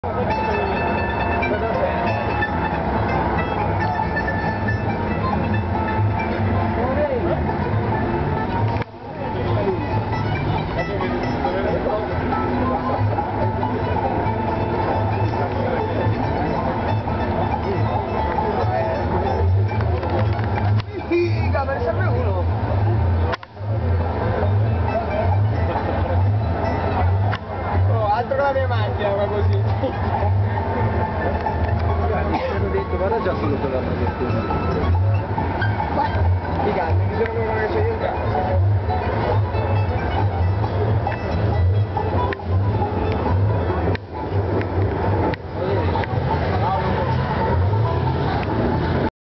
antonio mephisto presenta lo scania longline di Roland Singer al week end del camionista 2009
antonio mephisto presenta lo scania longline di roland al week end del camionista 2009